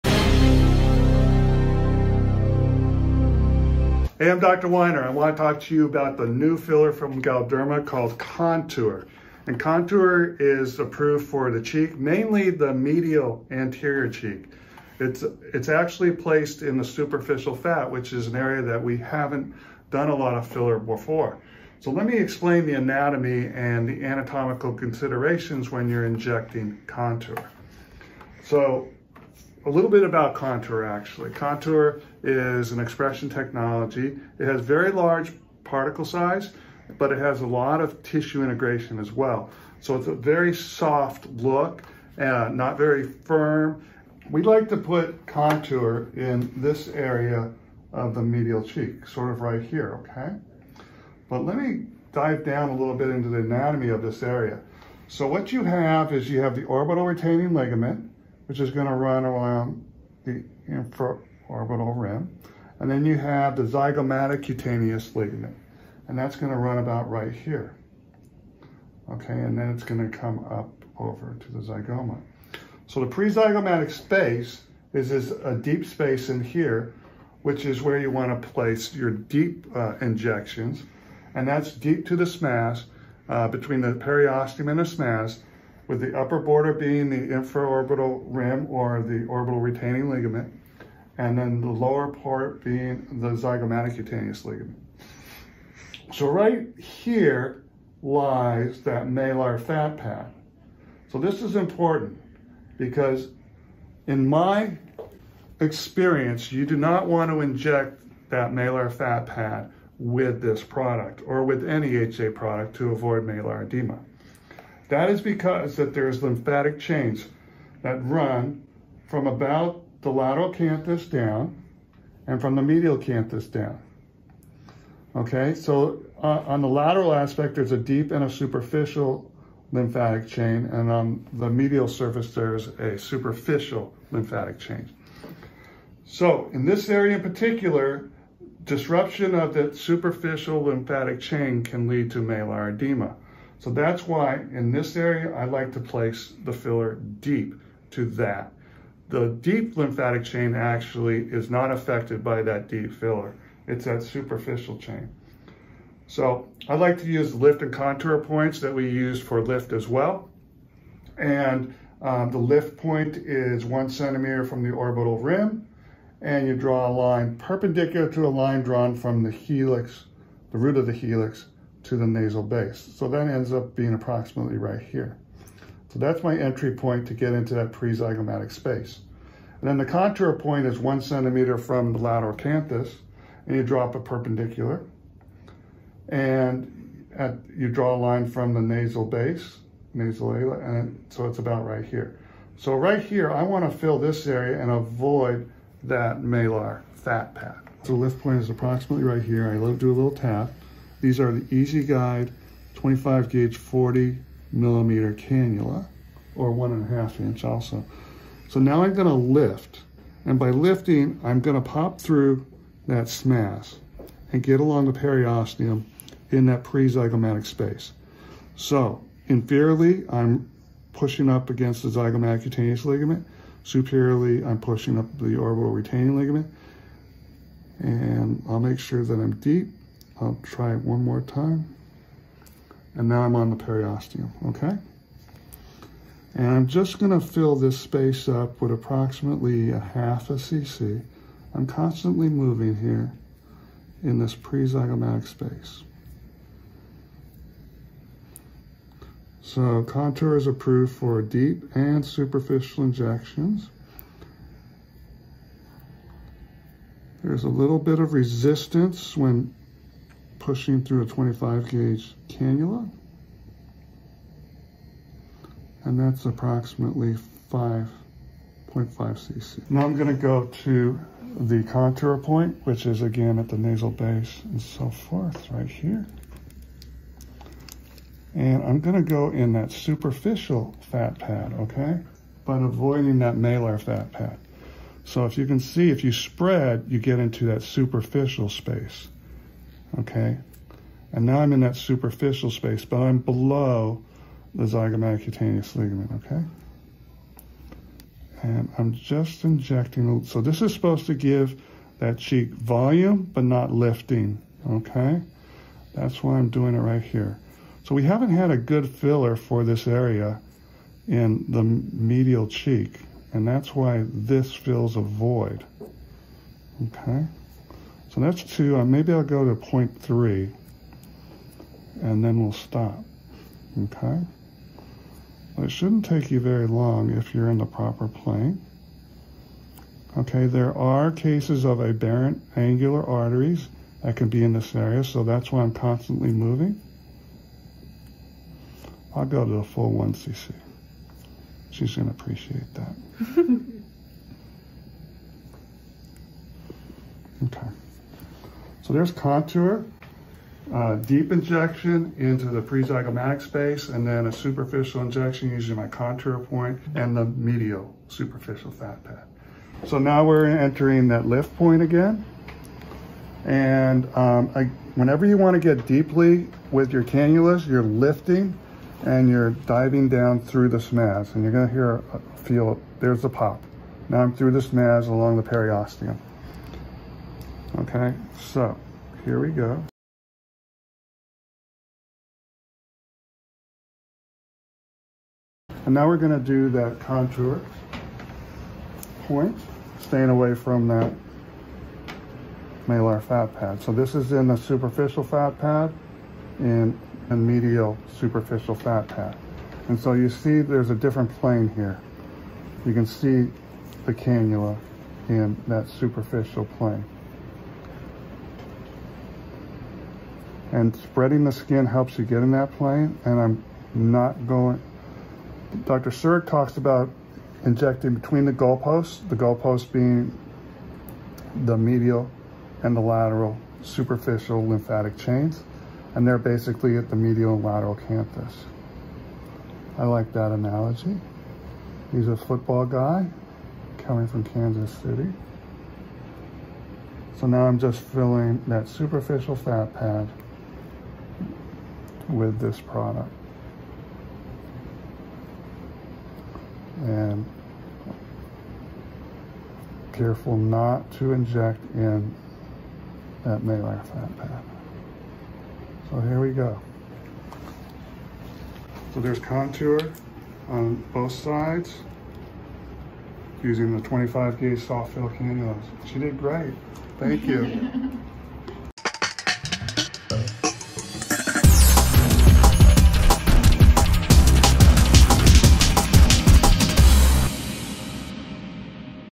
Dr. Weiner Shares His Injection Technique On The NEW Restylane Contour
Contour is shown being placed in the prezygomatic space and the superficial fat pads of the cheek. Dr. Weiner explains the pertinent anatomy and cautions against placement in the malar fat pad. There are also lymphatic channels, which need to be avoided during injection. This new filler can provide anterior projection when placed superficially without visibly seeing the product due to its very high tissue integration. Every filler has its unique characteristics and rheology and it’s up to the injector to choose the best filler and plane to optimize patient outcomes. “Remember the SMAS is your friend” (Dr. Surek) and injections should be superficial or deep to the SMAS depending on location, safety concerns, and desired effect.